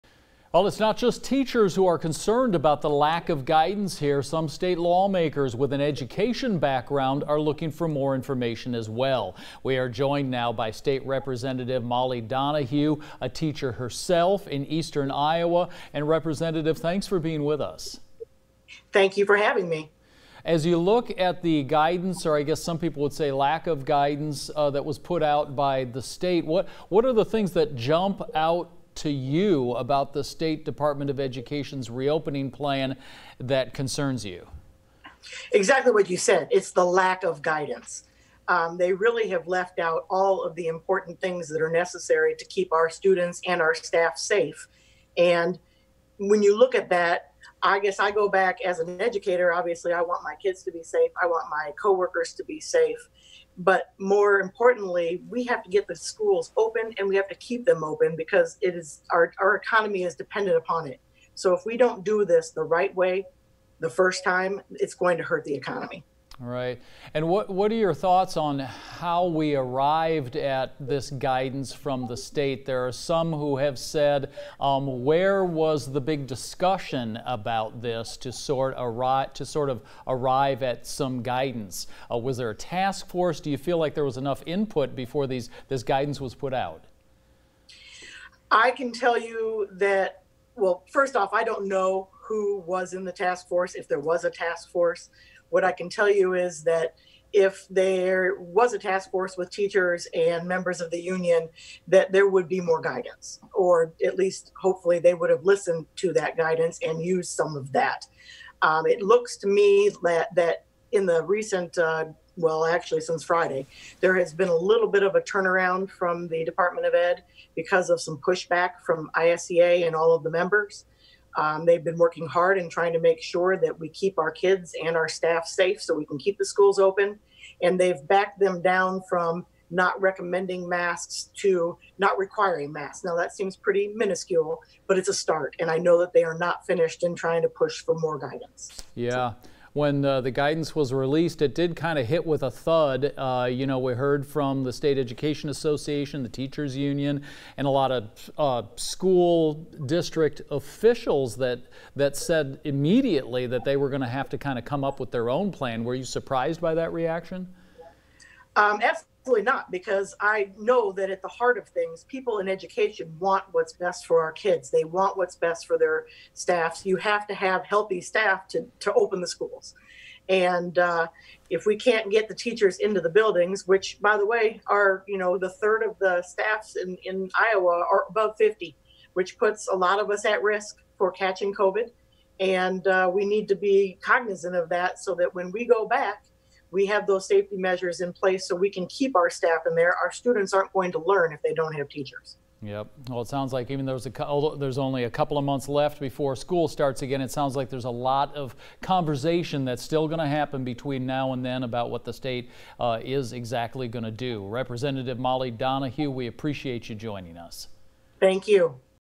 Rep. Molly Donahue on Iowa's "Return to Iowa" guidance
Rep. Molly Donahue, D-Cedar Rapids, talks with Local 5 about the Iowa Department of Education's "Return to Learn" guidance for schools.